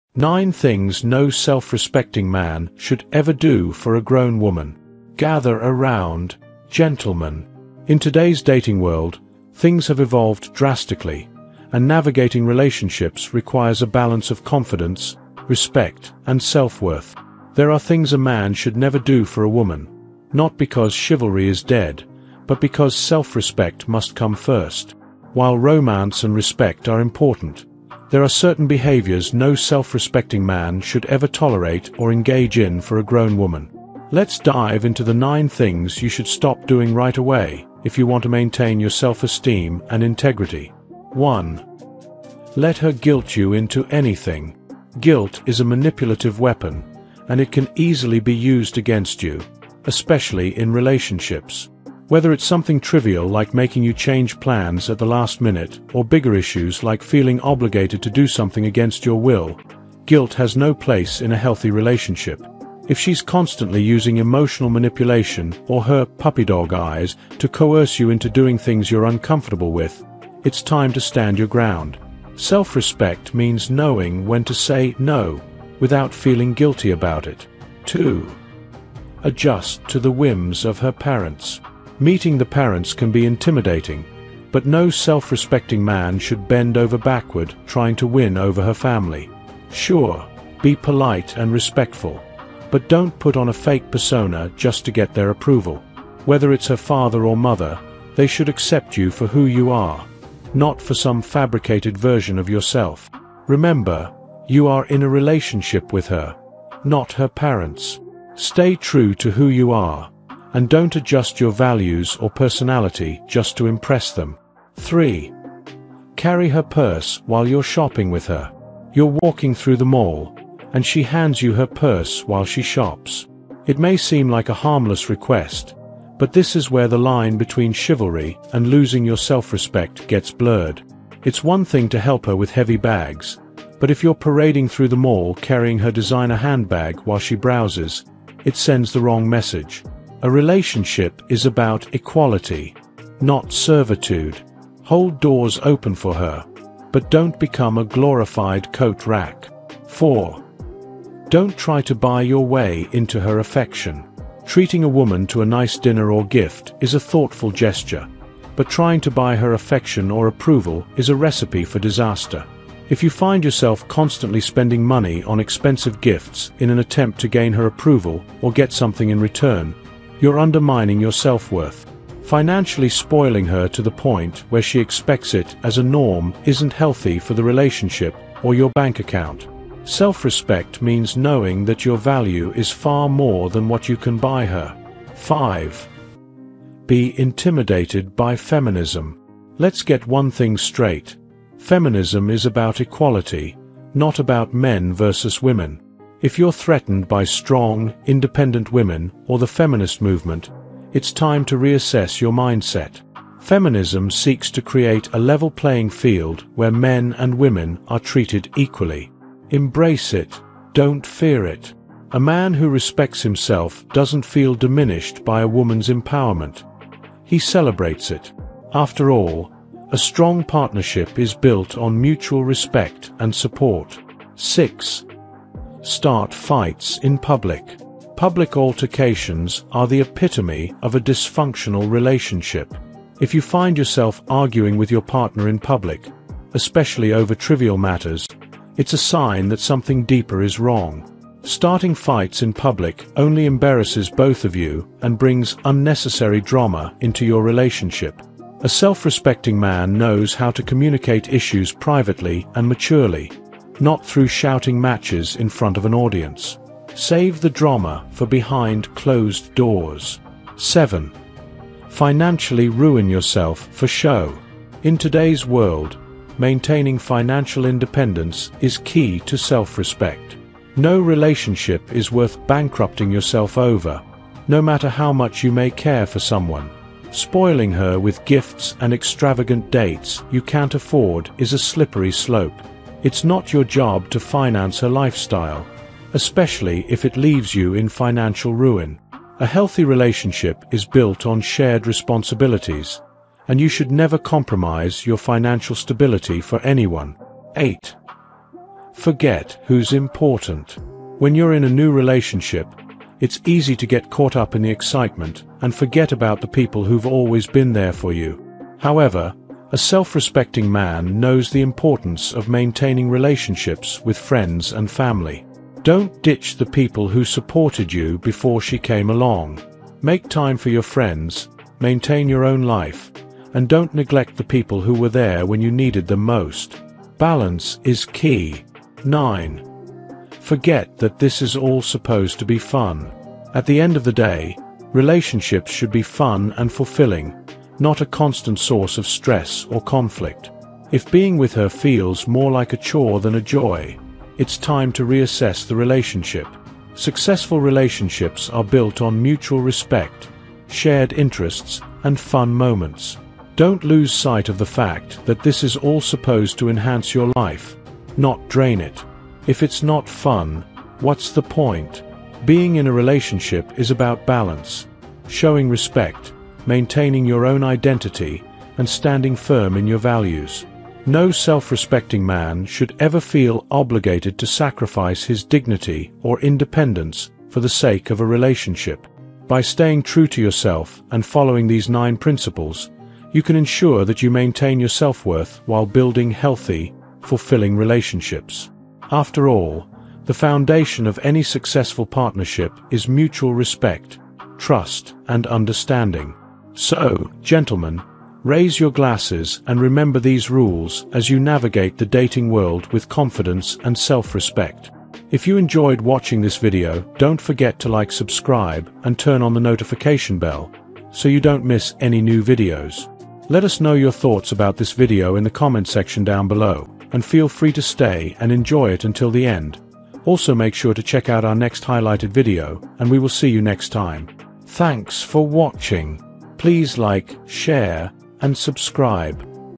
9 Things Men Should Never Do for Women: Essential Relationship Advice for Self-Respect | Super Facts
9 Things Men Should Never Do for Women: Essential Relationship Advice for Self-Respect | Super Facts
In every relationship, self-respect is key to maintaining a healthy balance. As men, it's important to know where to draw the line and stand up for your values and boundaries. In this video, we explore the 9 things that no self-respecting man should ever do for a grown woman. These aren't just dating tips; they're essential rules for every man who wants to build strong, meaningful connections while maintaining his own sense of worth.

We delve into topics like guilt trips, managing parental expectations, and why you should never carry her purse when you're out shopping together. It’s about understanding that respect in relationships should go both ways. If you’re constantly bending over backward to accommodate her whims, it can erode your confidence and self-esteem. Learn why adjusting to unreasonable demands, buying her affection, and letting her manipulate your time aren’t healthy behaviors for any man.

This video also covers how to handle feminist perspectives, the importance of keeping your cool in public arguments, and why you should never put yourself in financial jeopardy just to impress her. Successful relationships are based on mutual respect, and we explain how you can foster that while keeping your integrity intact.

You'll discover why it’s crucial to remember the people who’ve always supported you—your family and friends—and not lose sight of what’s truly important in life. Plus, we emphasize one final golden rule: relationships should be fun, not stressful! Women are amazing, and your bond with them should bring joy, not unnecessary worry.

Whether you're in a long-term relationship or just starting to date, these tips will help you navigate the sometimes tricky waters of love while keeping your self-worth in check. Watch until the end for practical advice that can change the way you approach relationships forever!